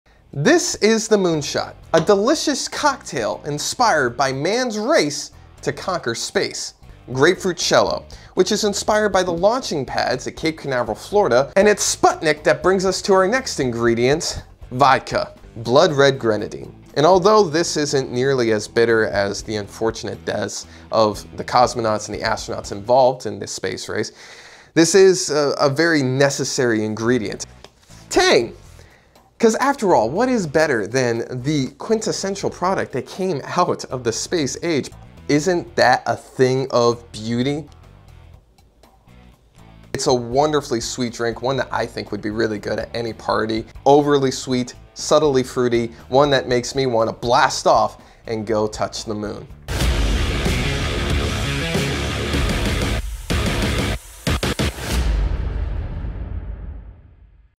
TAKES you to the MOON | Moonshot Cocktail
A shot that will take you to the moon. 🤴🏻SAVE this post! More recipes at TopShelfHistory.com

Ingredients:
* 3/4 oz Grapefruitcello
* 3/4 oz Russian Standard Vodka
* Splash of Grenadine
* 1 Scoop to Tang into 1/2 C of Water

First, mix 1 scoop of Tang into 1/2 C of water. Stir to mix.

Then, into a shaker, pour 3/4 oz of Grapefruitcello, 3/4 oz of Russian Standard and the 1/2 C of Tang. Stir all ingredients to mix.

Pour into a shooter glass, then add a splash of Grenadine.